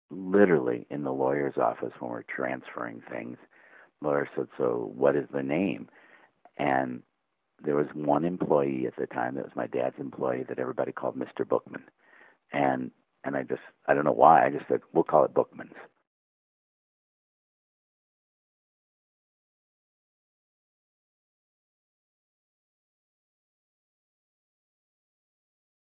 What's The Story Behind Bookmans Name?
Bob Oldfather, founder of Bookmans Entertainment Exchange, explains where the name came from.